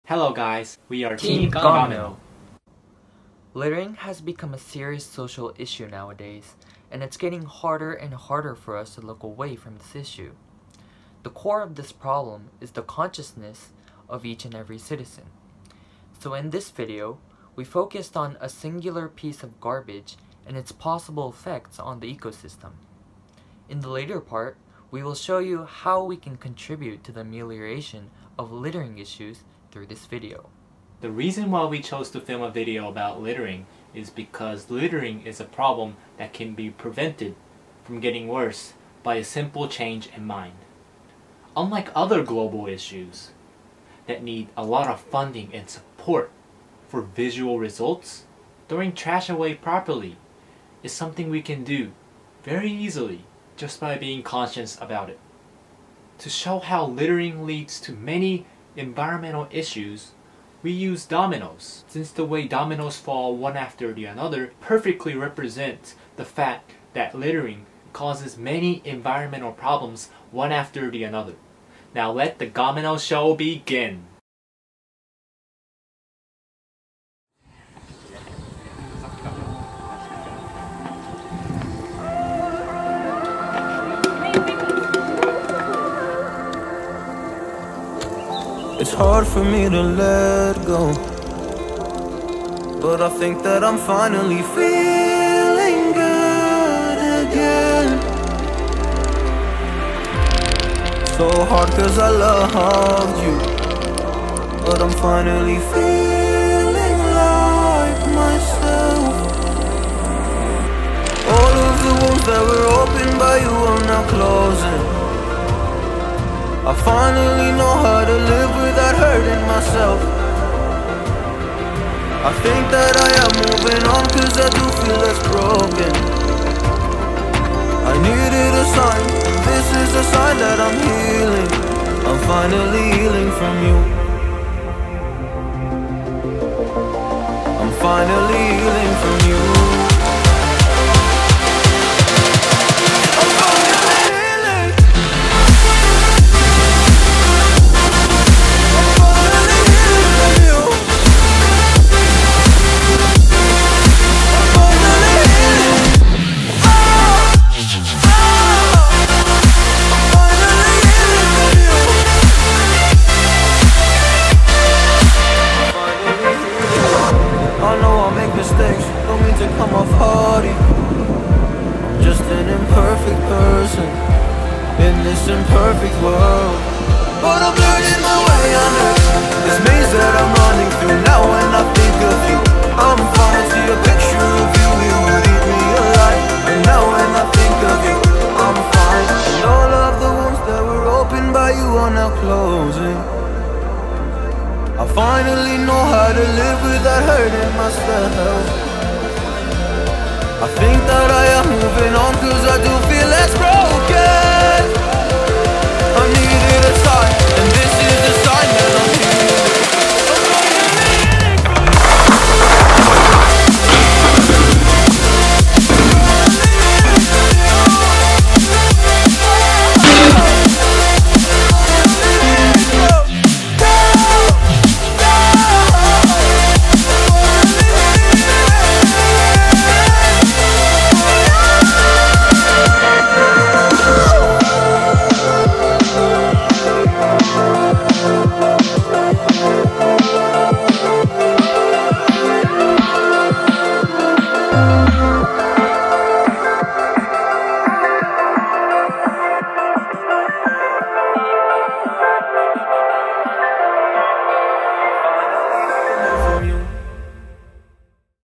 Gomi No! - JALT PIE conference video by Team Gomi No! - Yuta, Shou , Oichiro, Takuma, & Kouta
Video made by Yuta Aihara, Shou Kiyotaka, Oichiro Sera, Takuma Miyoshi, & Kouta Henmi
Littering has become a serious social issue nowadays, and it is getting harder and harder for us to look away from this issue. The core of this  problem is the consciousness of each and every citizen. In this video, we focused on  “a singular “ piece of garbage and its possible effects on the ecosystem. In the latter part, we will show you how we can contribute to the amelioration of  littering issues through this video. The reason why we chose to film a video about littering is because we thought that littering is a problem that can be prevented from getting worse by a simple change in mind. Compared to other global issues that need lots of funding for visible results , throwing trash away properly is something we can do easily by just being conscious about it. To show how littering leads to environmental problems, we used dominoes, since the way dominoes fall one to another, perfectly represents the way littering causes environmental problems one after the another.